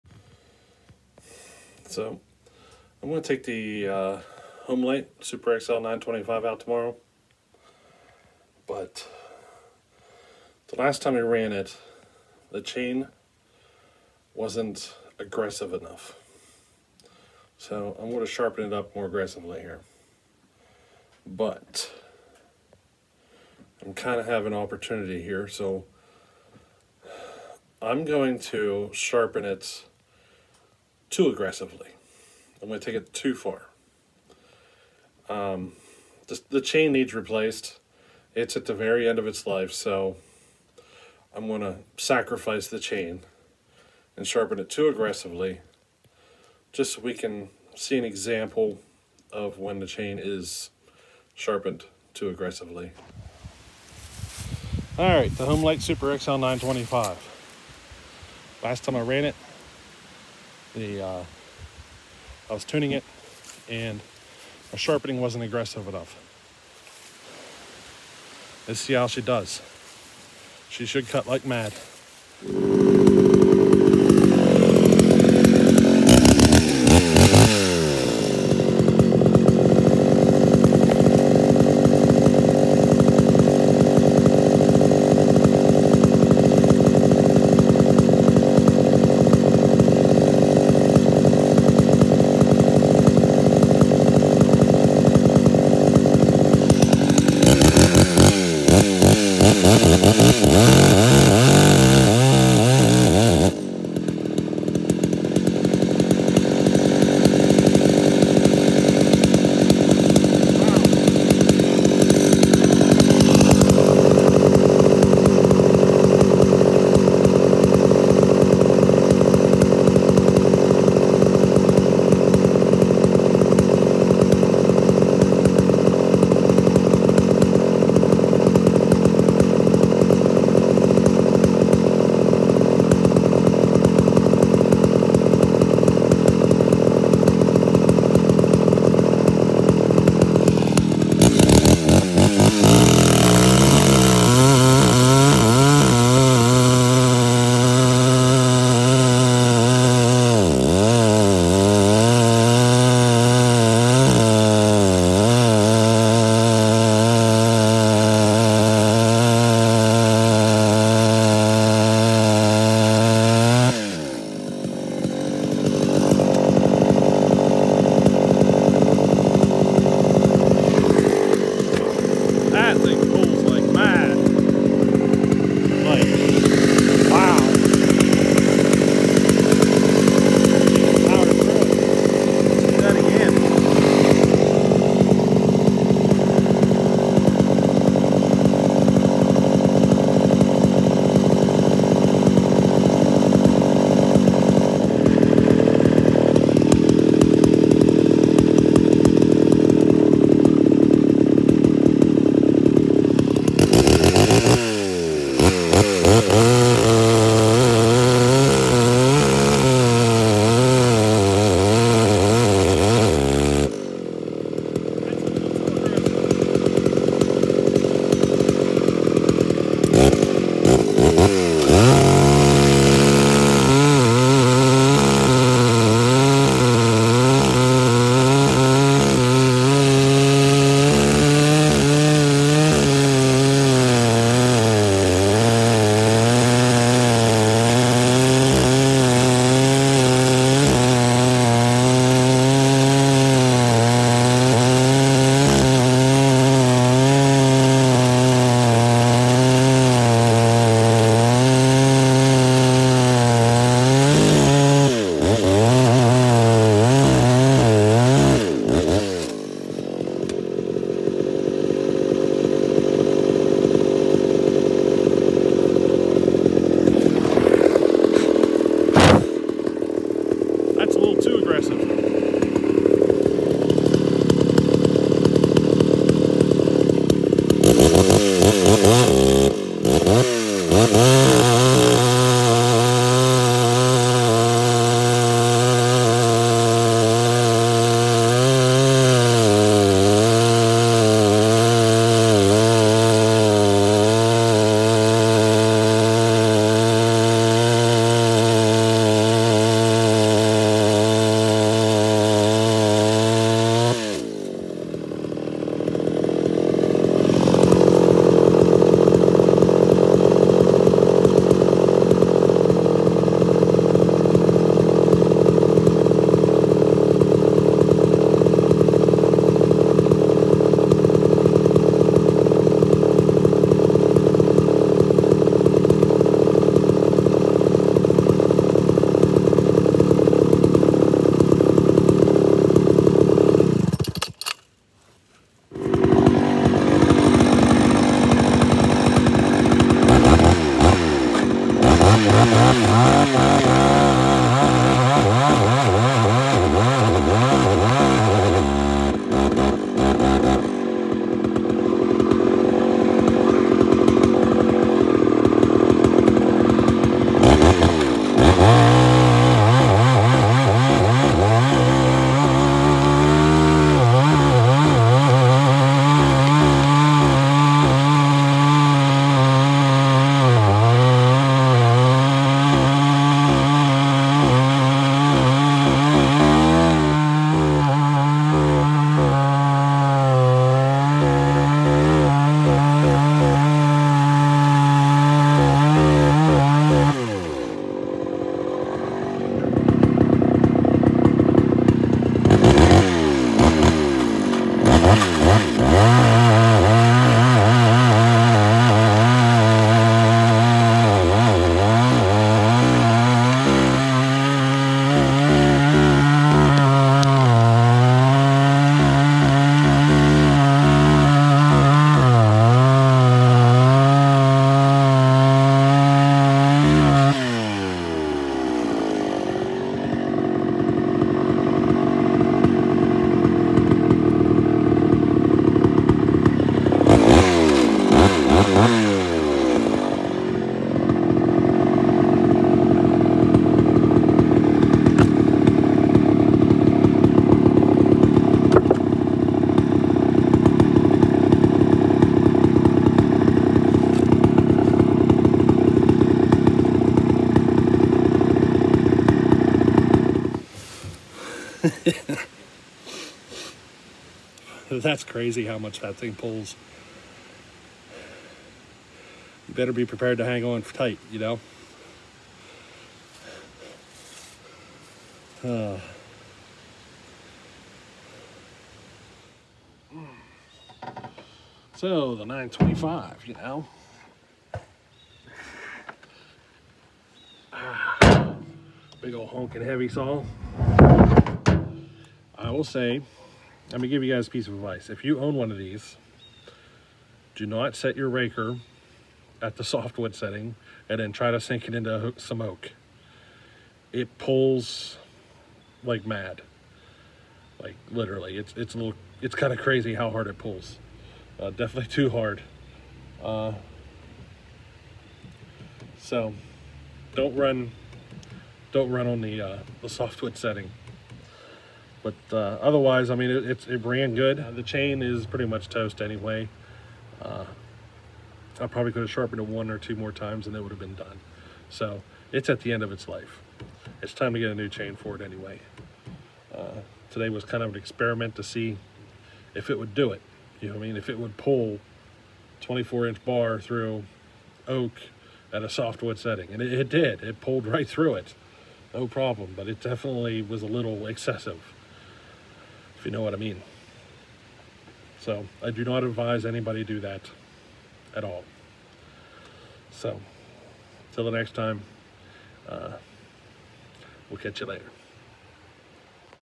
Your Chainsaw Is To Sharp!! Homelite Super XL-925 82cc!!
This is one example of how to tell if you chainsaw is sharpened to aggressively.